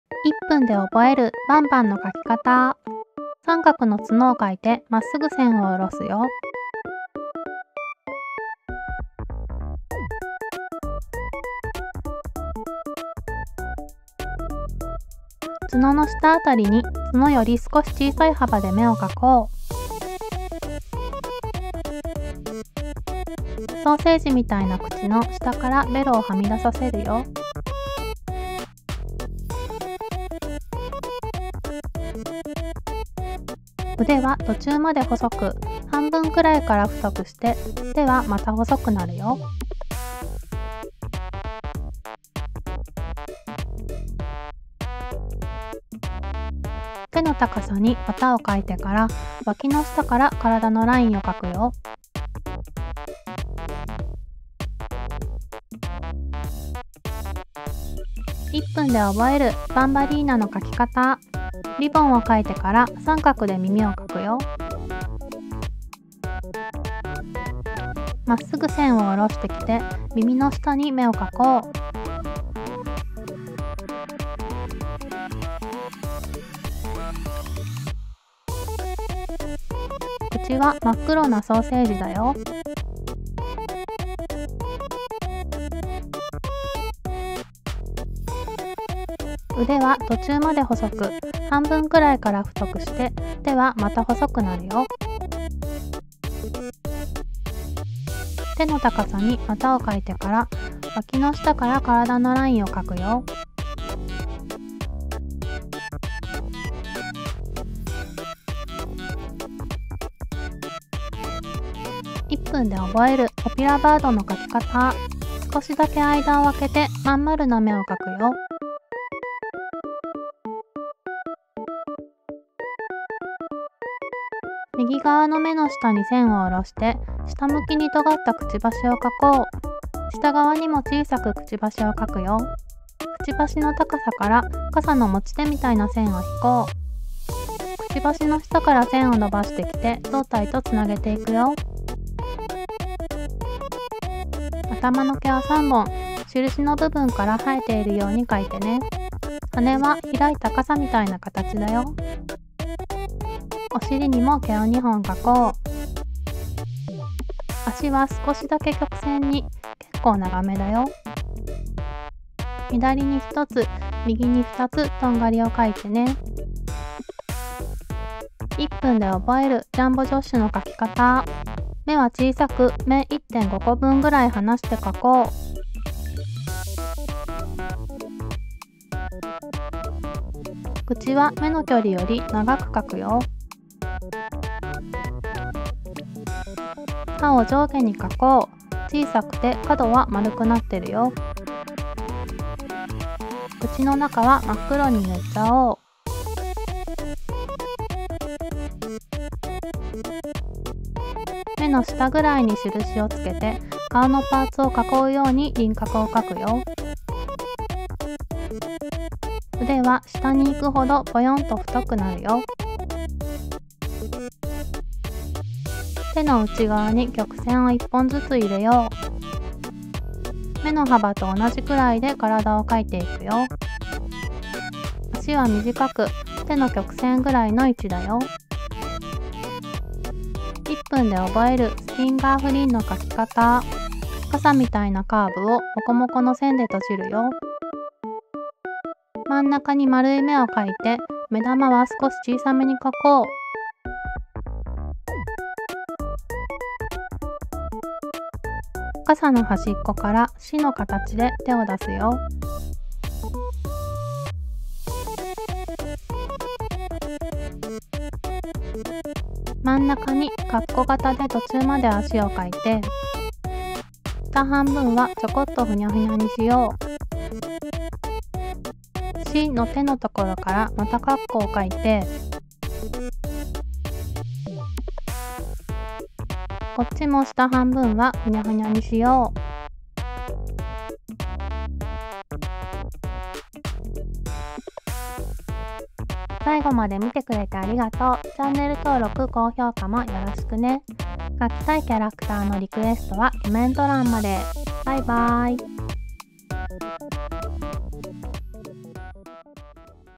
【イラスト】こどもも描ける！？ガーデンオブバンバンのキャラの描き方を解説！How to draw【GARTEN of BANBAN】gartenofbanban
バンバン  0:00
バンバリーナ  0:58
オピラバード  1:52
ジャンボジョッシュ 2:48
スティンガーフリン   3:50
★描きたいキャラクターのリクエストはコメント欄へ！★

「まもの動画で描いてみた」で
描いたお絵描きツイートしてみてね♪見に行きます！